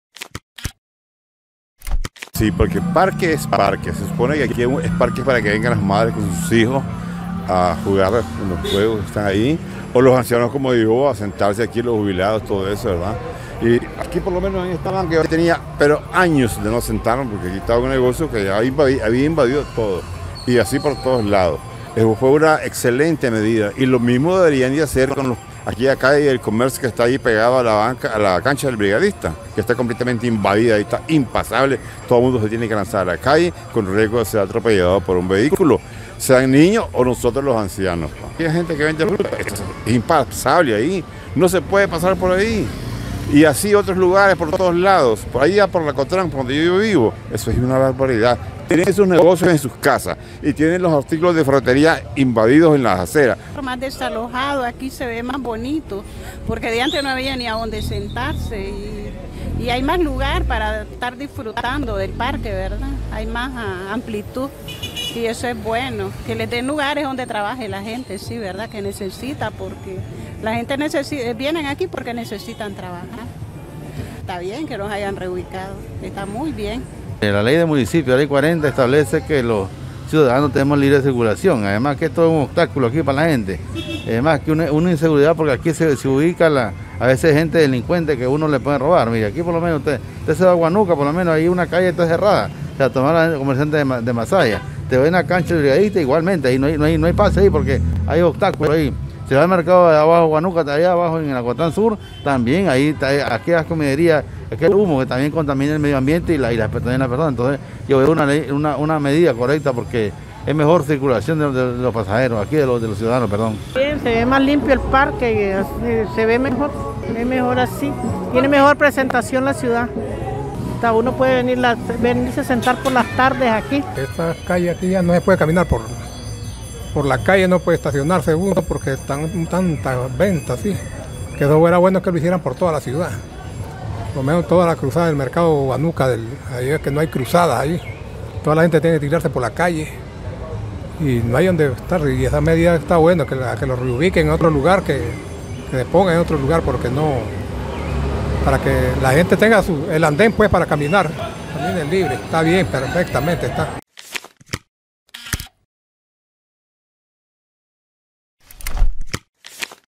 Conformes con ordenamiento de espacios públicos en Matagalpa
Habitantes de Matagalpa valoran positivo el ordenamiento de espacios públicos en la ciudad. mosaicocsi.com